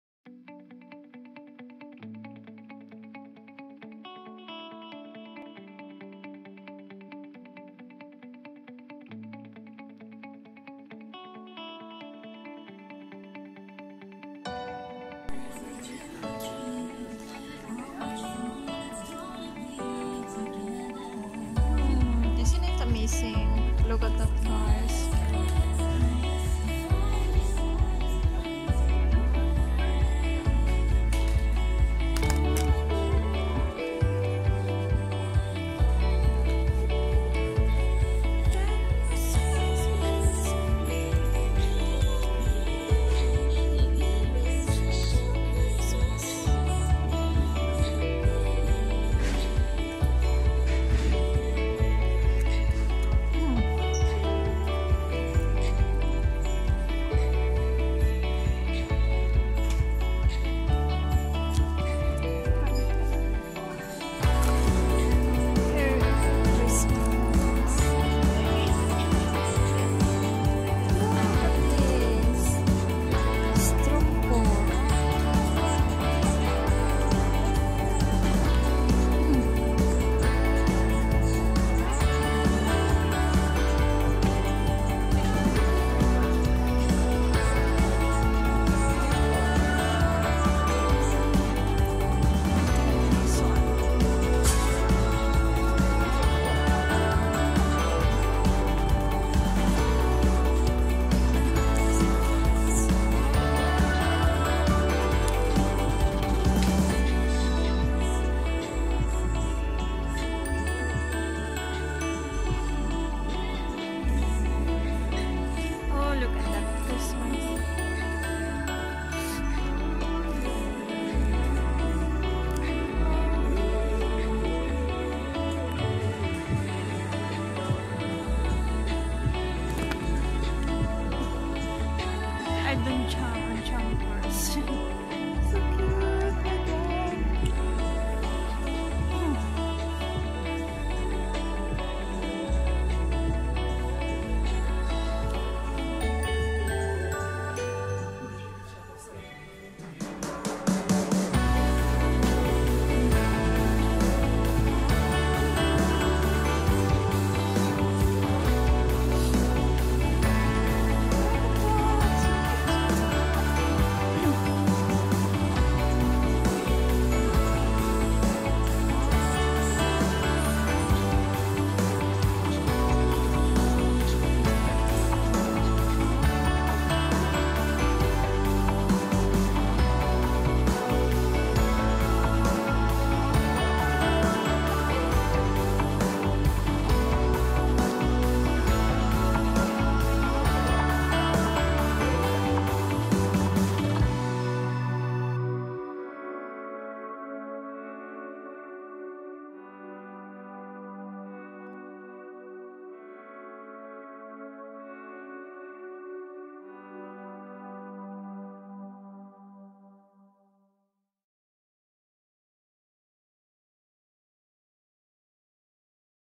Christmas in Ullared // by Amies family vlog
Shopping mall Ullared
Ullared is a biggest shopping mall or store in sweden
Ullared has many  things and products with the affordable prices
Many people go there and shop many specially they live from far away